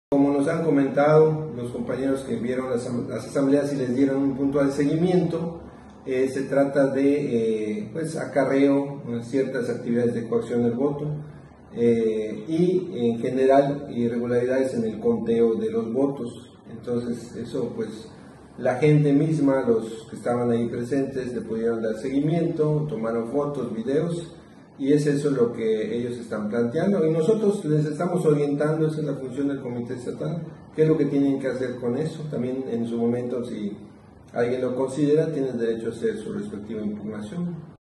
Morenistas confirman irregularidades en las recientes elecciones internas para consejeros estatales.
Mérida, Yucatán, 3 de agosto de 2022.- Acarreo de participantes en autobuses, coacción del voto e irregularidades en el conteo de votos, se registraron durante las elecciones a consejeros distritales del partido Movimiento de Regeneración Nacional (Morena) en Yucatán, acusa el presidente del Comité Ejecutivo Estatal, Mario Mex Albornoz.

De acuerdo con Mex Albornoz, varios de sus compañeros de partido dieron seguimiento a las irregularidades mencionadas durante los comicios internos de Morena, las cuales dijo, fueron con fotos y videos.

“Nosotros, en el Comité Estatal, les estamos orientando sobre qué es lo que tienen que hacer con eso; también, en su momento, si alguien lo considera tienen derecho a hacer su respectiva impugnación” aseveró el morenista.

Con ello, el presidente del Comité Ejecutivo Estatal de Morena evidencía las fracturas que existen al interior del partido en el poder, por lo que podrían surgir mayores conflictos entre los integrantes del partido guinda, en caso de que los afiliados y participantes de la reciente elección interna presenten la impugnación y la respalden con las pruebas correspondientes.

Hay que recordar que, el domingo pasado militantes del partido guinda acusaron prácticas irregulares ejecutadas por ex integrantes del Partido Revolucionario Institucional (PRI), quienes  ya se han integrado a Morena, lo cual derivó en conflictos, así como dimes y diretes entre quienes las diferentes fracciones de ese partido.

Cabe resaltar que, de acuerdo con cifras de Morena, en las elecciones para consejeros en Yucatán del domingo pasado participaron únicamente 20 mil personas, lo cual muestra una baja participación entre sus militantes, si se compara con las 357 mil personas  que fueron parte de la consulta sobre revocación de mandato el pasado mes de abril.